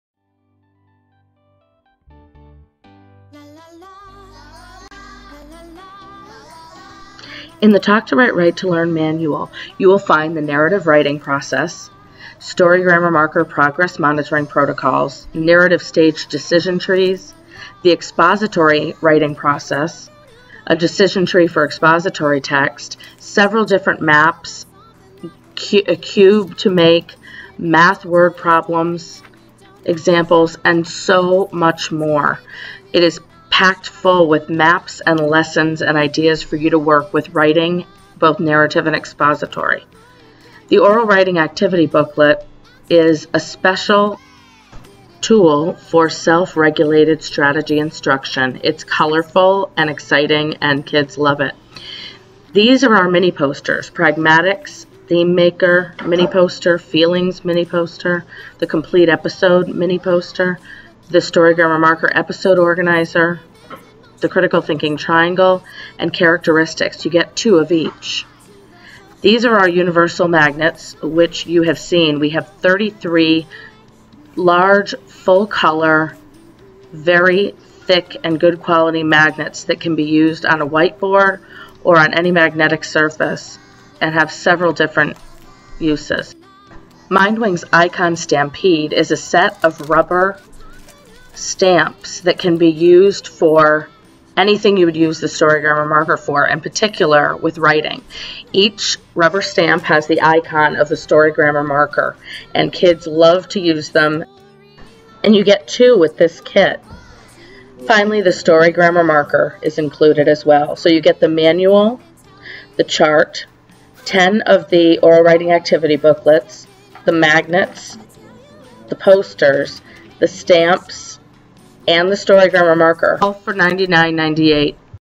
Talk To Write, Write to Learn Kit 50% OFF - Deal of the Day - Dec 11 2017
Holiday Countdown Deal of the Day for Monday, Dec 11, 2017: the Talk to Write, Write to Learn Kit for Narrative and Expository Text Writing - 50% off! (Reg. $199.95, get it today for $99.98!!!)
Deals Change Every day at 8AM, now through December 21. to shop!